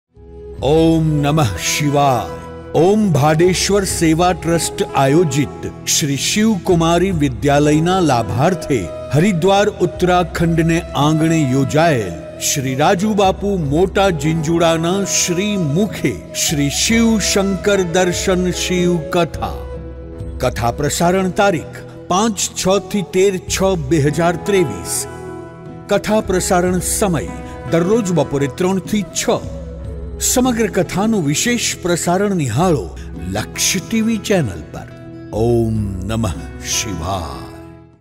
હરિદ્વાર ઉત્તરાખંડના આંગણે યોજાયેલ | શ્રી શિવ શંકર દર્શન "શ્રી શિવ કથા" | D-Live
હરિદ્વાર ઉત્તરાખંડના આંગણે યોજાયેલ

શ્રી શિવ શંકર દર્શન "શ્રી શિવ કથા"

વક્તા : શિવકથાકાર શ્રી રાજુબાપુ (મોટા જીંજુડા)

વિશેષ પ્રસારણ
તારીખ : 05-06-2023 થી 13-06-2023
કથા પ્રસારણ સમય : બપોરે 03:30 થી 06:30 કલાકે

વિશેષ પ્રસારણ : લક્ષય ટીવી